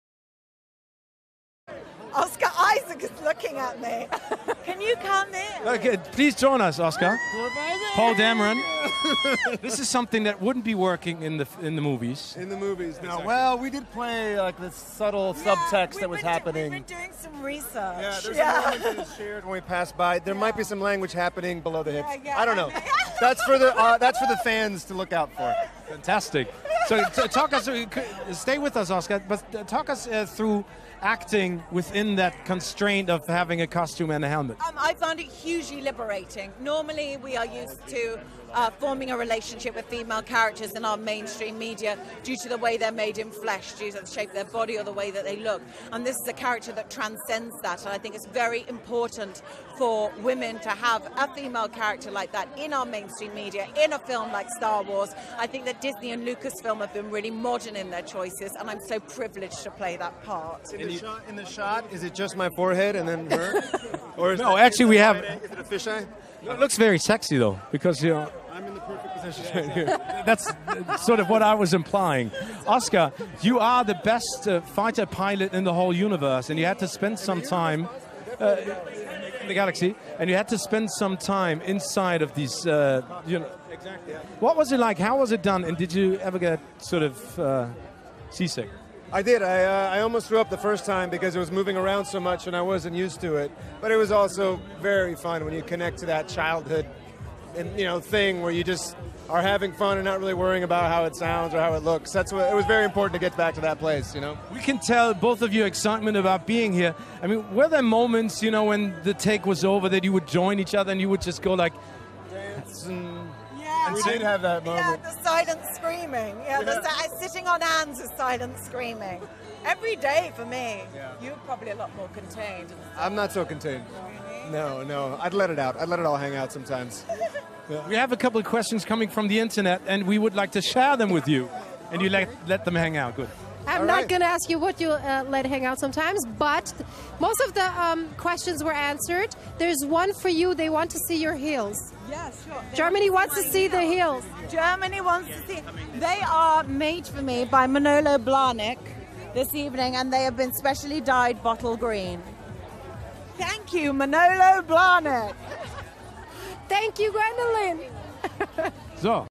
Interview: Gwendoline Christie and Oscar Isaac at The Force Awakens European Premiere
Star Wars: The Force Awakens - Now playing in theaters.